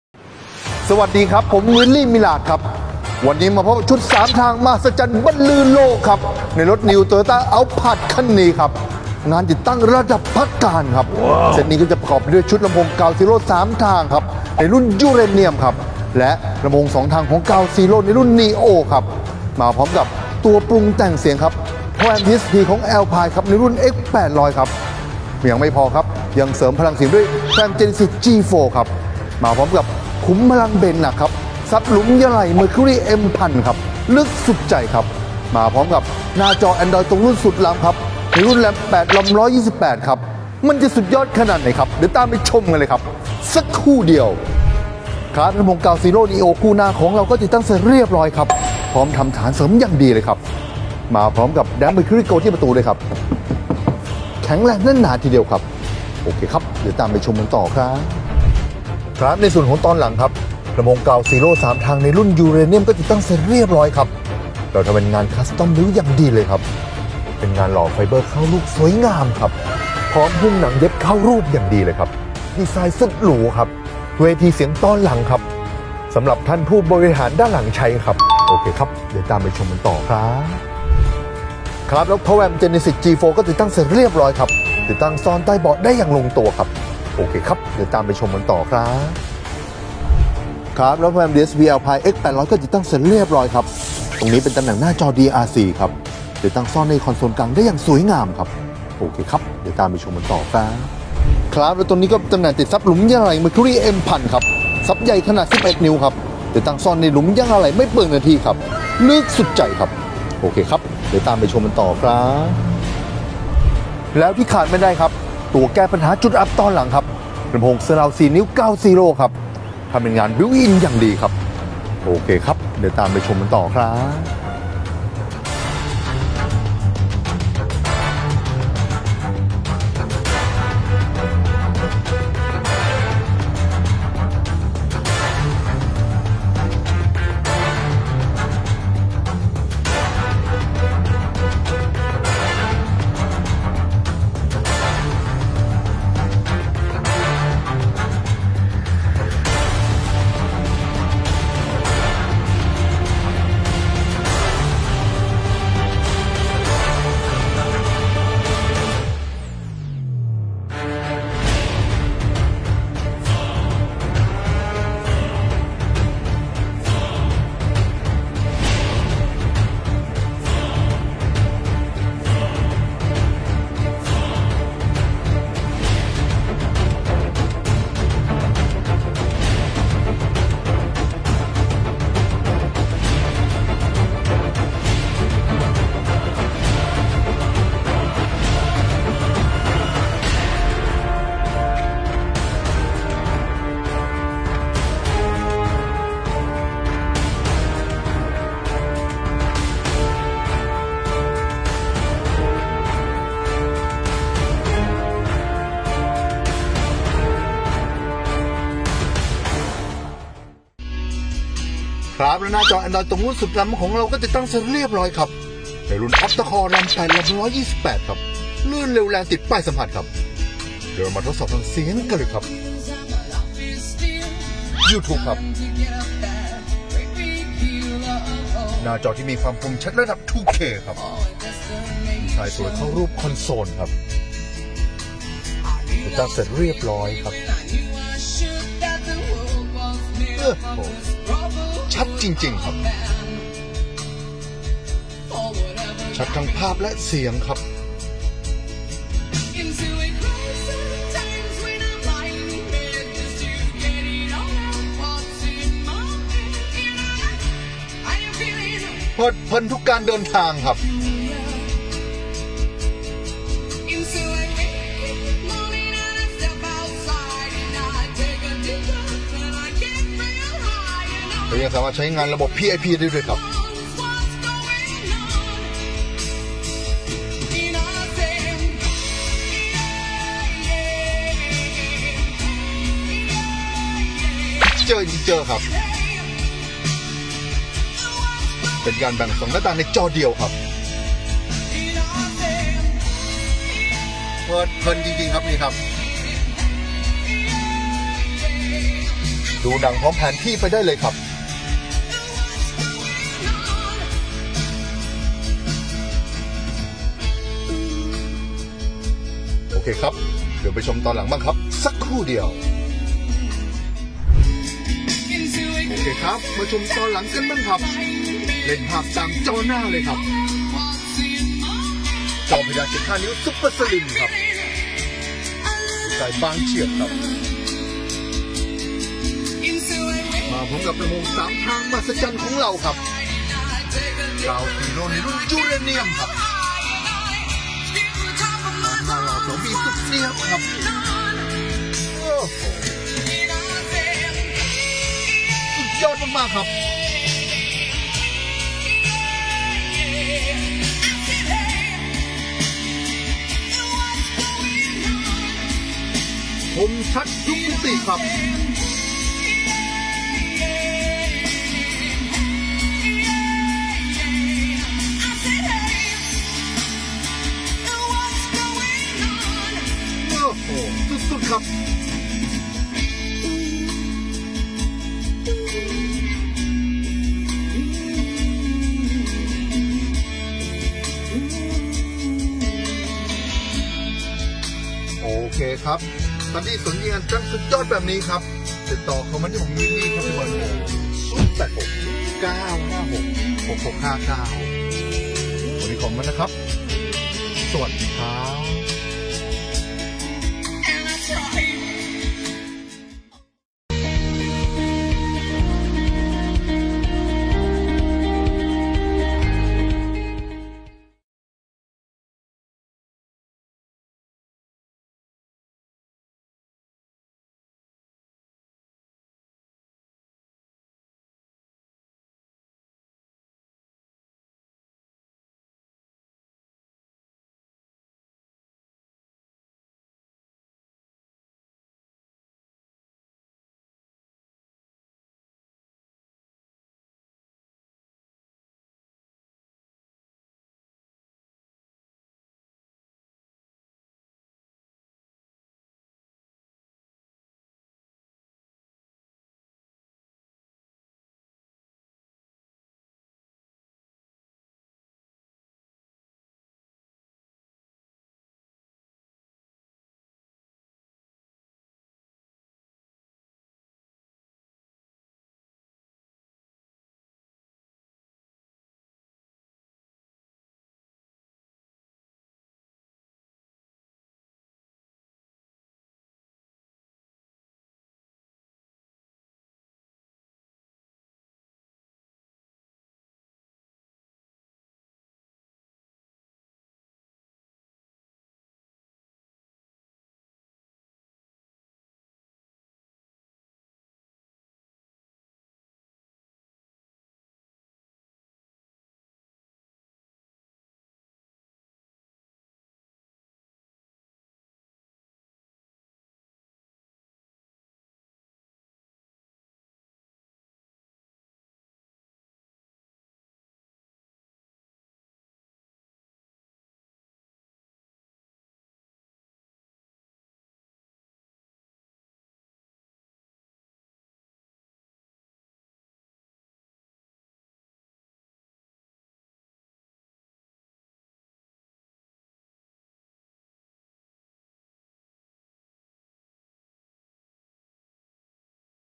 Toyota Alphard เครื่องเสียงชุด 3ทางมหัศจรรย์บันลือโลกจอAndroid ตรงรุ่น by Willy Mirage
Toyota Alphard เครื่องเสียงชุด 3 ทางมหัศจรรย์บันลือโลก พร้อม จอ Android ตรงรุ่น by Willy Mirage
🌟 SPEC
✅ Ground Zero Uranium 3 ทาง
✅ Ground Zero Neo
✅ Power Amp DSP ALPINE PXE-X800
✅ Power Amp Genesis G4
✅ Mercury M1000
✅ Damp Mercury Gold
✅ หน้าจอ Android ตรงรุ่นสุดล้ำ
🔸 OCTA - CORE - RAM 8 / ROM 128
🔸 รองรับแอพพลิเคชั่น Youtube / Netflix / Google Maps / Spotify / Google Play
🔸 ระบบ PIP Picture in Picture เป็นการแบ่ง 2 หน้าต่างในจอเดียว